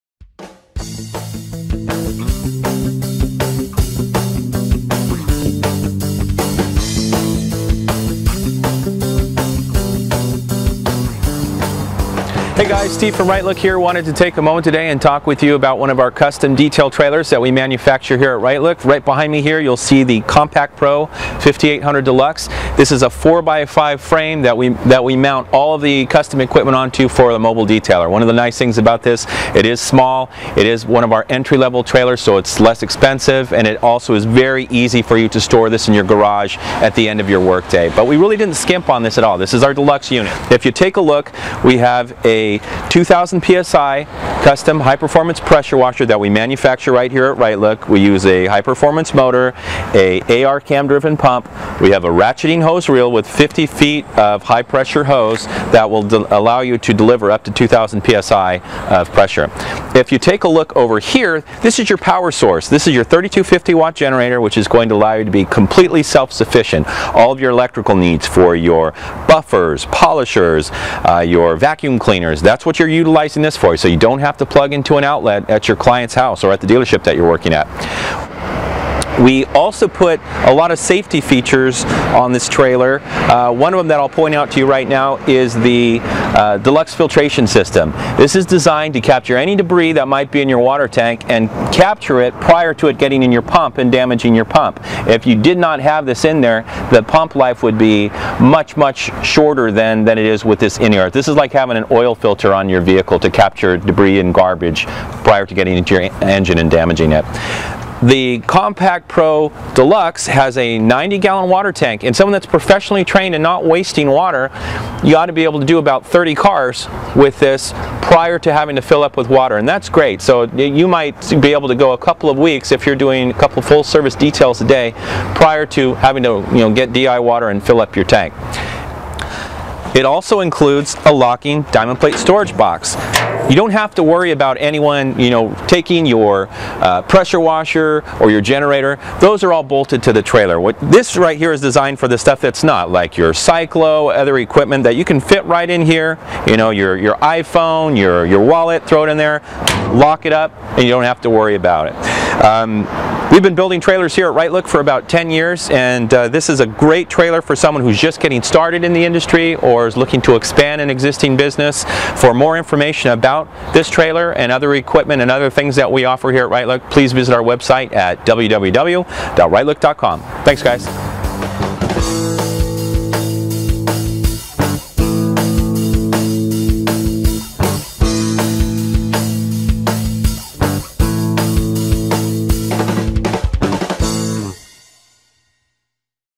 Mobile Auto Detailing Trailer: Compact Pro 5800 Deluxe with Steve Drennon
Rightlook.com offers the best Mobile Auto Detailing Trailers for both entry level and experienced professional auto detailers. The Compact Pro 5800 Deluxe mobile auto detailing trailer is the perfect trailer for someone just getting started in the industry or looking to expand an exisiting business.

to see more Mobile Auto Detailing Trailers Rightlook.com has to offer.